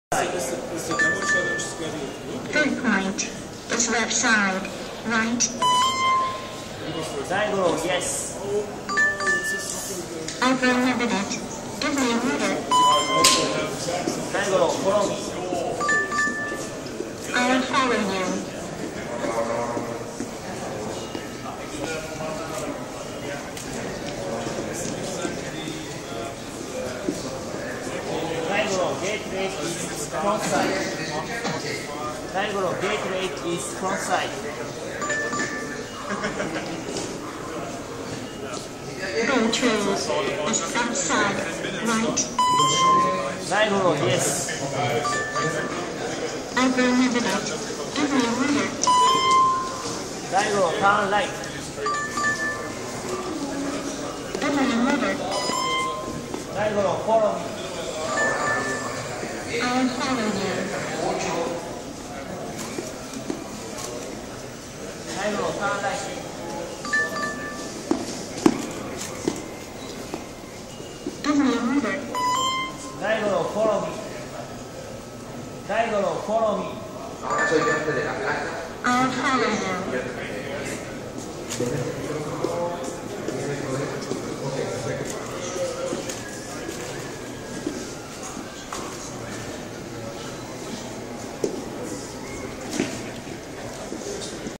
Digoro! Yes! - eR@sers - RoboCup@home 2012 - Mexico City
The eR@sers Team from Japan is testing their Robot Digoro. I love the sounds.